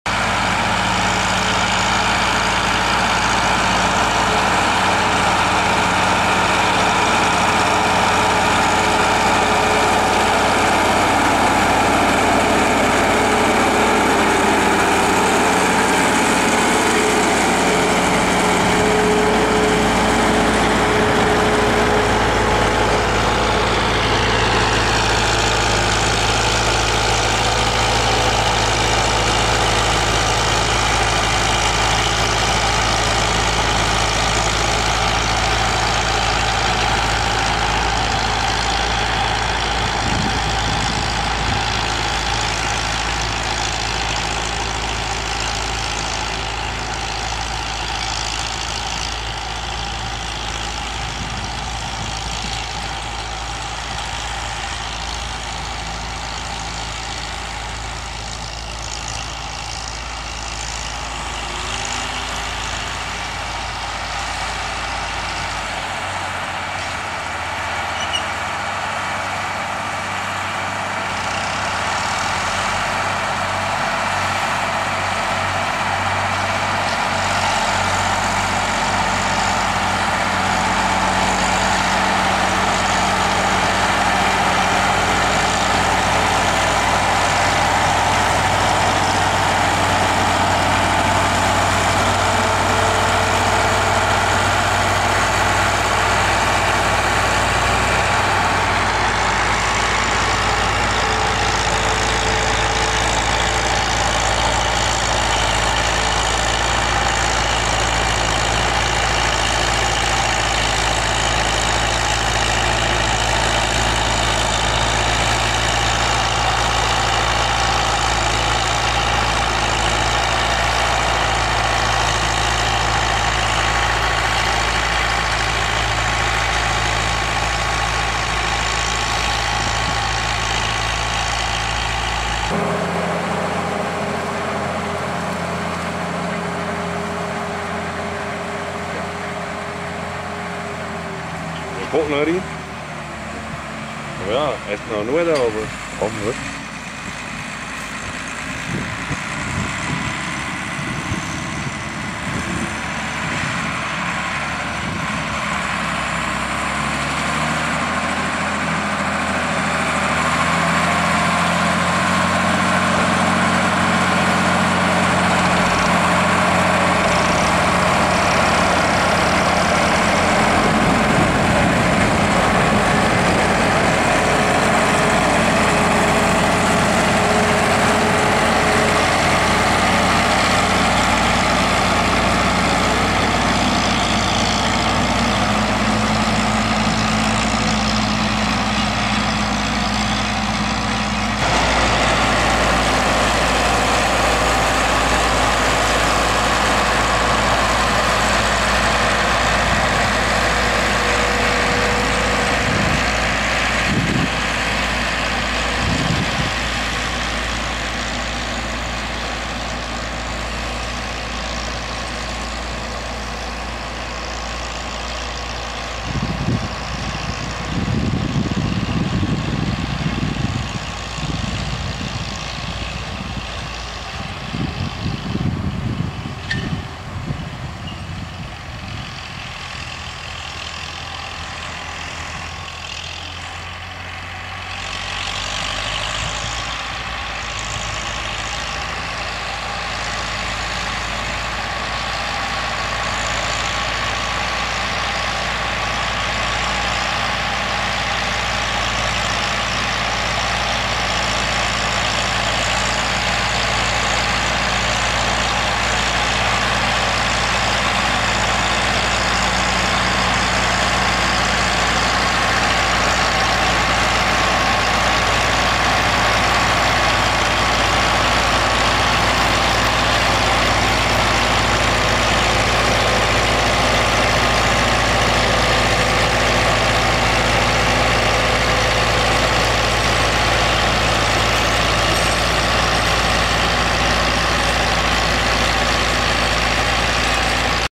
Landb. Playsier & Waeyaert - cultiveren - Valmet Volvo 2105 - Volvo BM T 650
Landbouwer Playsier uit Torhout zij aan zij met landbouwer Waeyaert uit Aartrijke aan het cultiveren met een Valmet Volvo 2105 en een Volvo BM T 650.